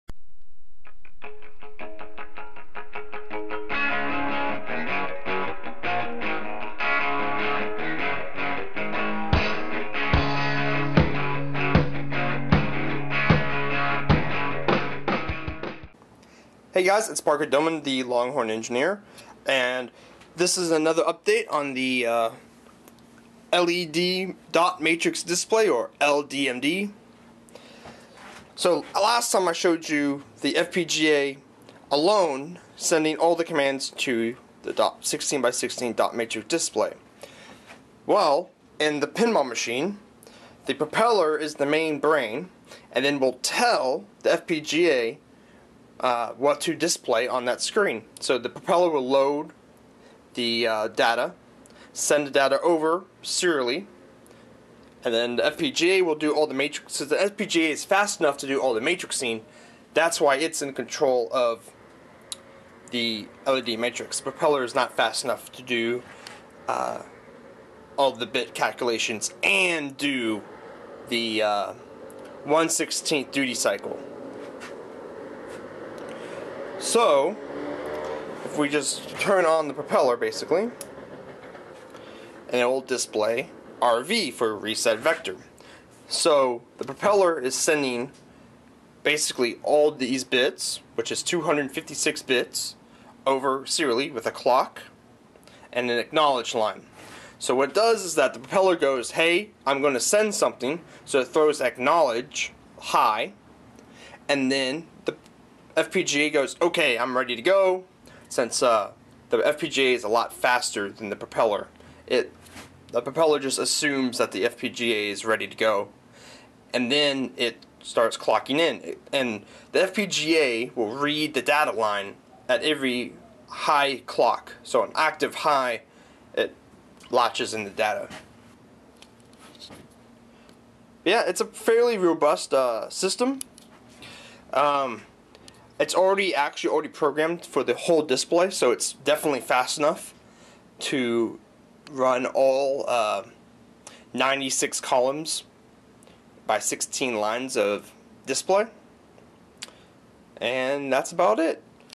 RESET VECTOR: LDMD 16x16 Propeller Communication Test
I have the Propeller to FPGA communication protocol written and tested. This is probably the most complicated thing I have designed and built so far. You can download the code I used below.

Here are some updated pictures of RESET VECTOR